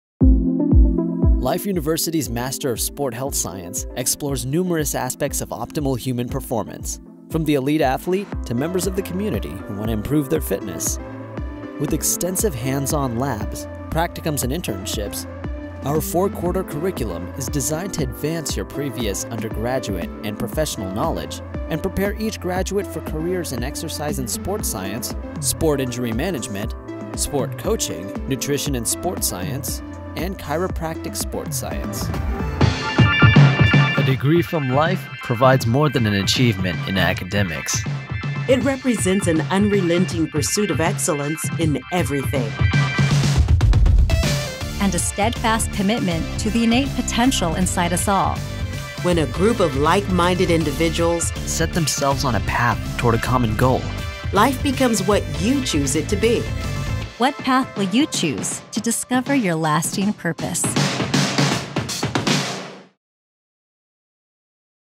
M.S. Sport Health Science
Life University offers a 52-credit hour Master of Science degree in Sport Health Science with specialty tracks in the professional fields of Exercise and Sport Science, Sport Injury Management, Sport Coaching, Nutrition and Sport Science, and Chiropractic Sport Science. The curriculum is designed to permit graduates with an interest in these specific areas to realize their personal and professional goals.